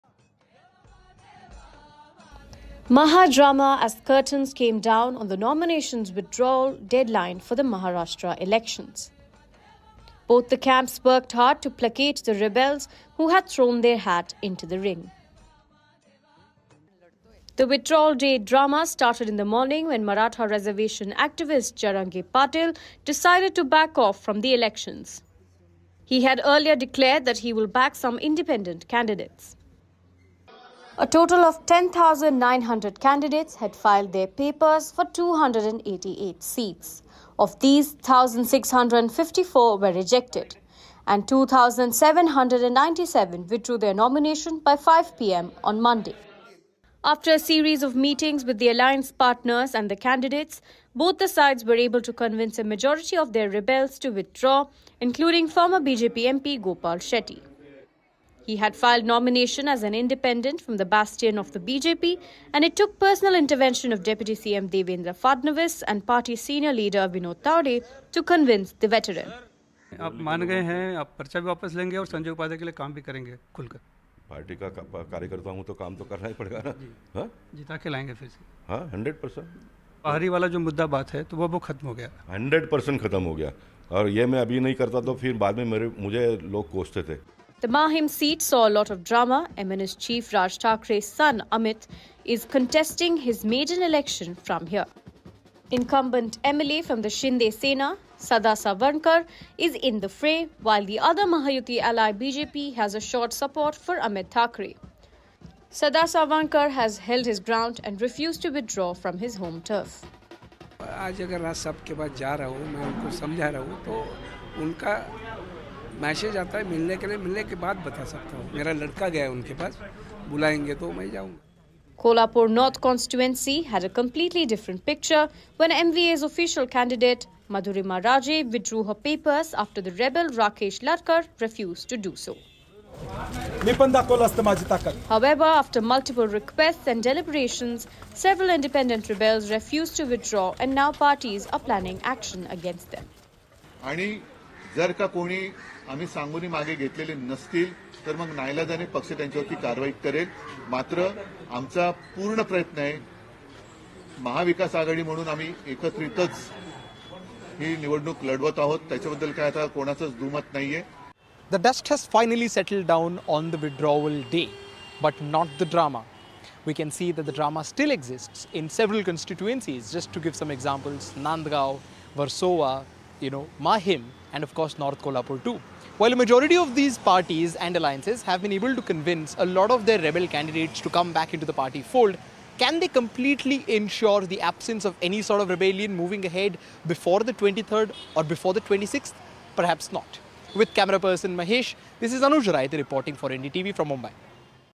Maharashtra Elections | Rebellion In Ranks As Maharashtra Elections Near
Ahead of the November 20 Maharashtra elections, major political parties are facing a rebellion in their ranks as party workers who were denied tickets defied their leaderships and filed nominations. Both the ruling Mahayuti and opposition Maha Vikas Aghadi worked hard to placate the rebels, who had thrown their hat into the ring.